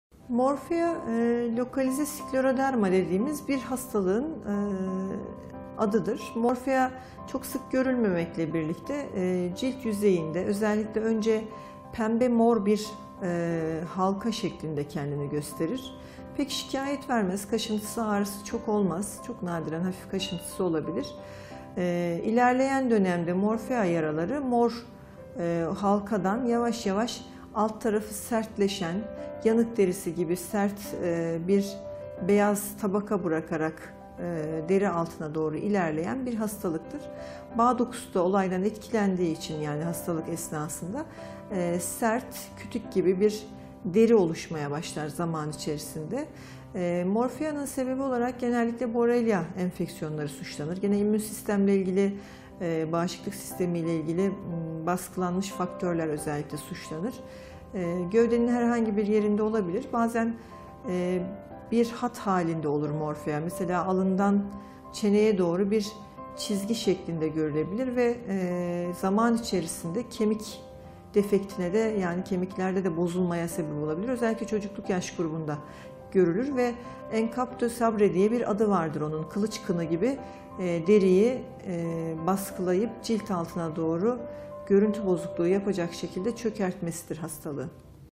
Morfea Hastalığı Nedir? Belirtileri Nelerdir?
Morfea Hastalığı Nedir?  Belirtileri Nelerdir? Uzm.Dr. Ayşegül BAYKAN  sizin için cevaplıyor.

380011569